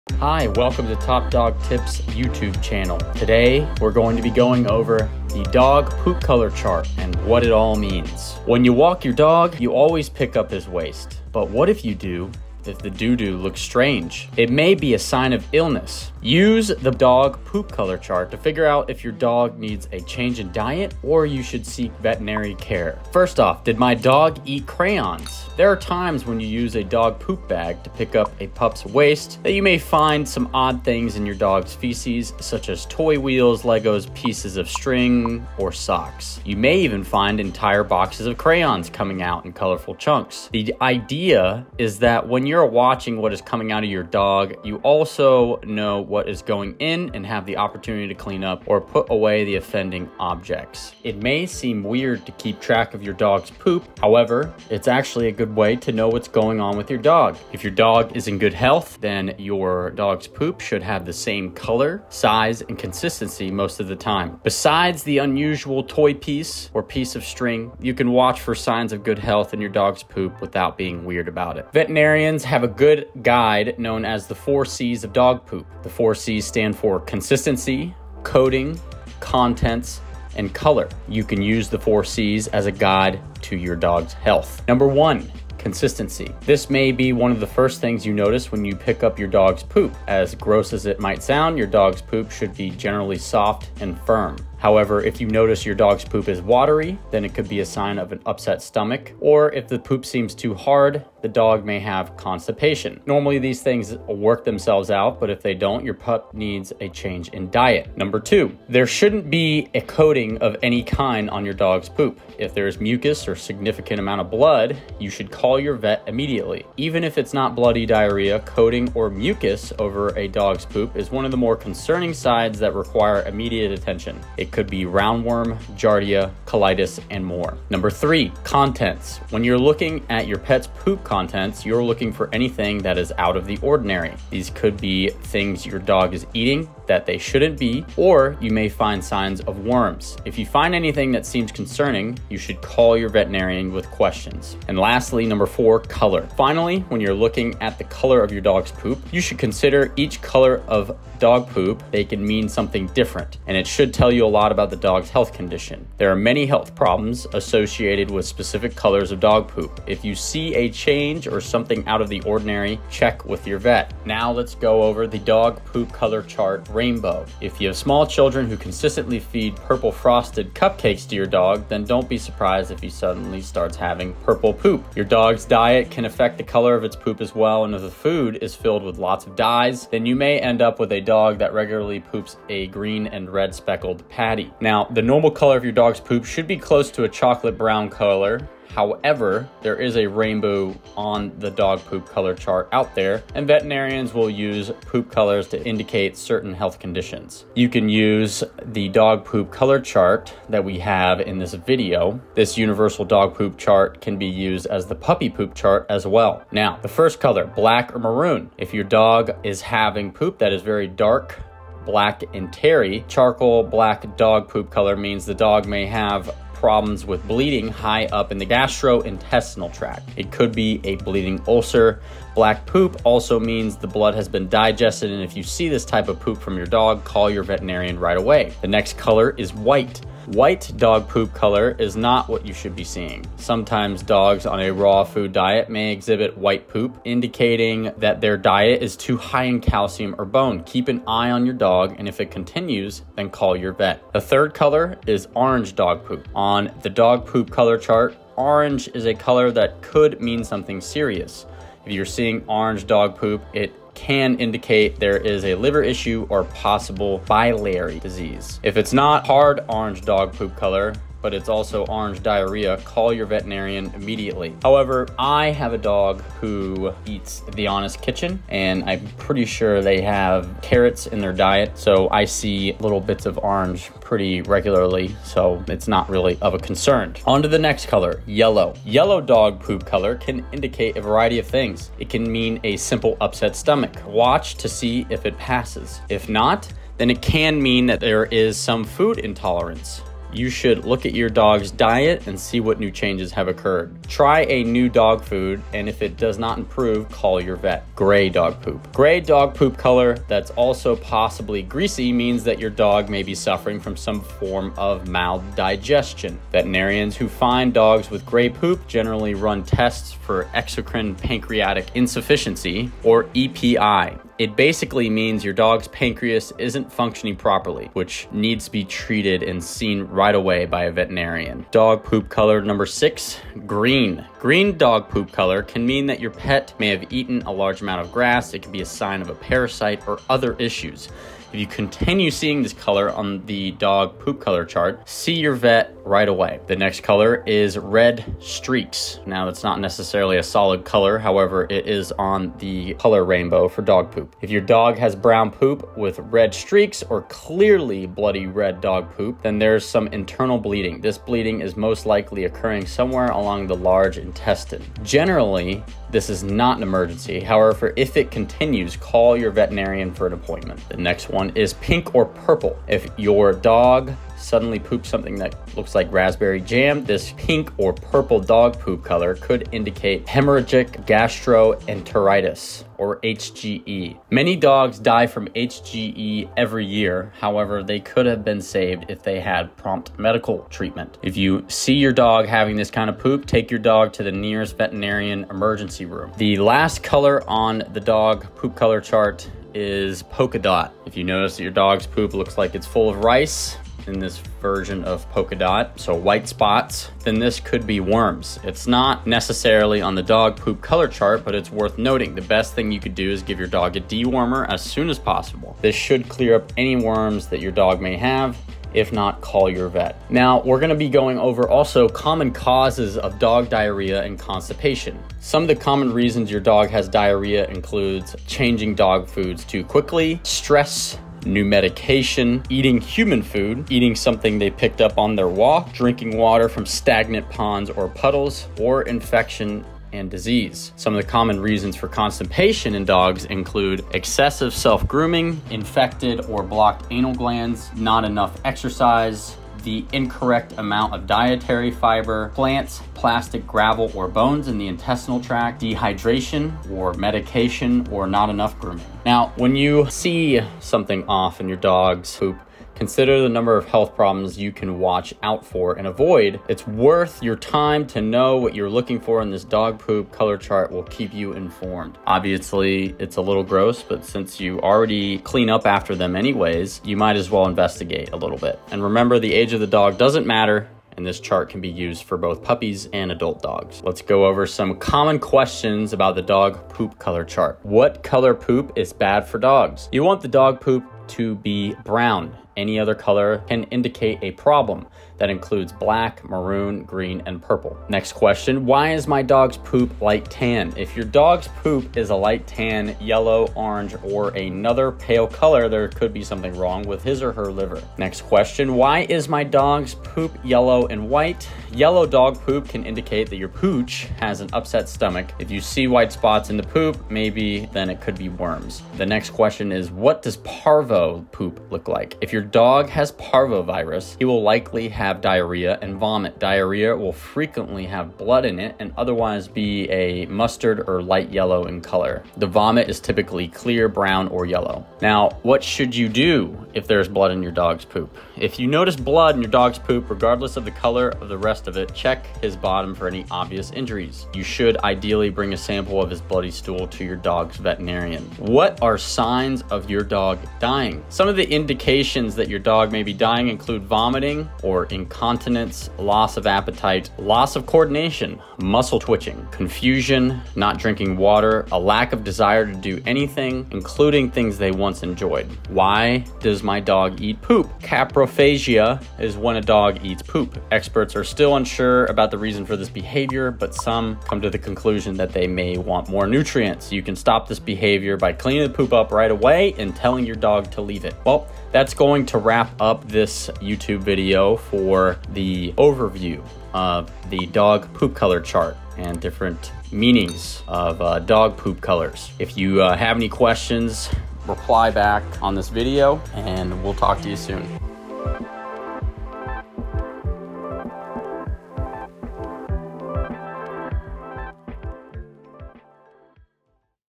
Dog Poop Color Chart: What It All Means
Today we are going to be talking about the dog poop color chart.